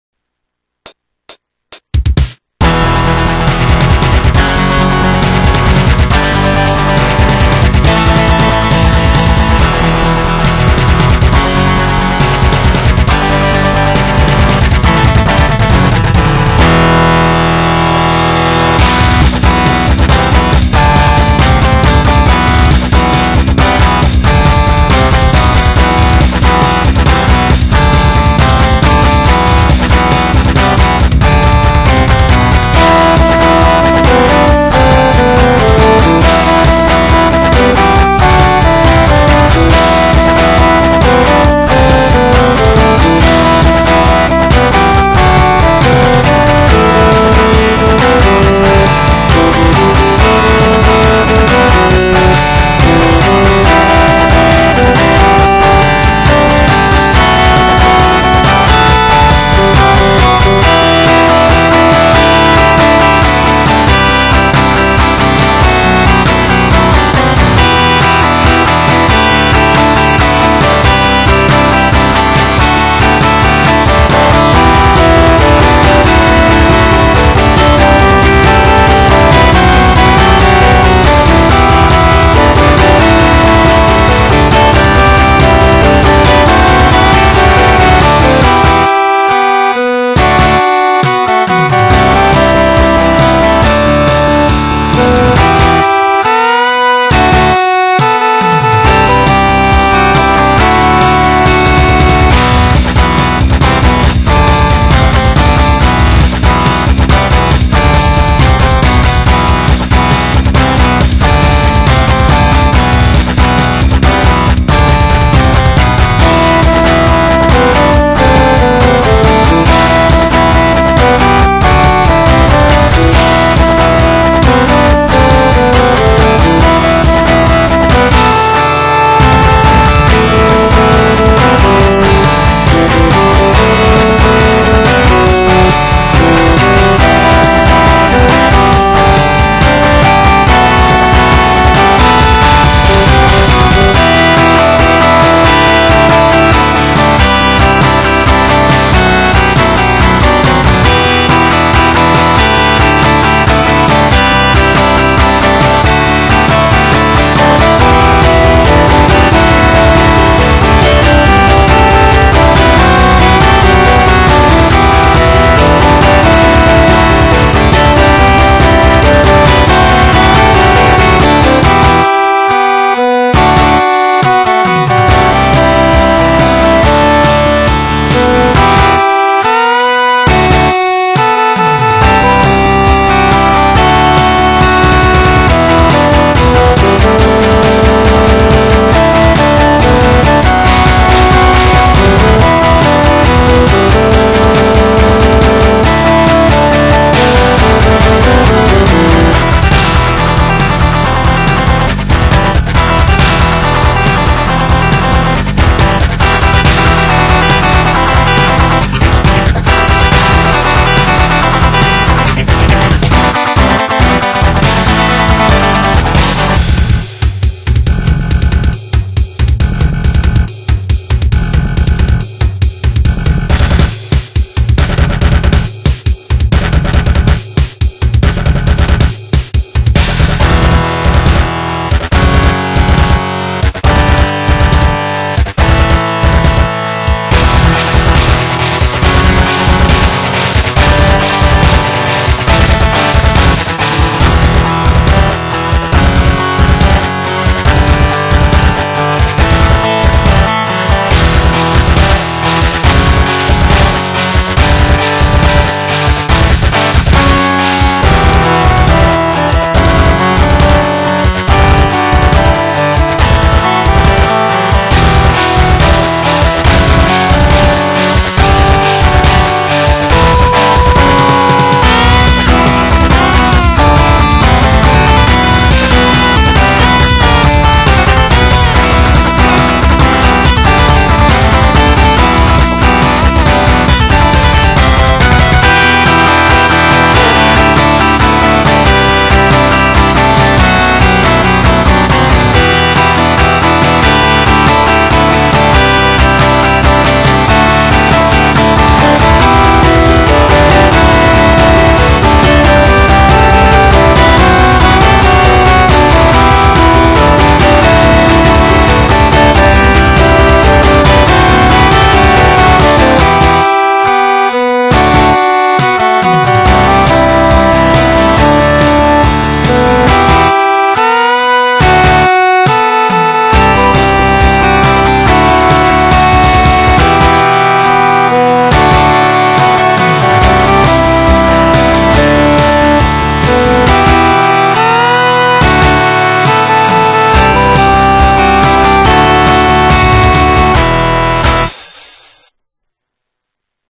Original Metal Song (TuxGuitar/Guitar Pro)
Sorry guys, I'm using Guitar Pro to compose this song...
Haven't name this song yet, no idea...
Actually this song is made by my brother...
Soon will be played by Real Instruments...

Guitar Pro 5 Instruments:
Distortion Guitar - Lead 1
Distortion Guitar - Lead 2
Distortion Guitar - Guitar Solo
Distortion Guitar - Rhythm
Soprano Sax - Vocal Melody
Slap Bass 2
Piano
Percussion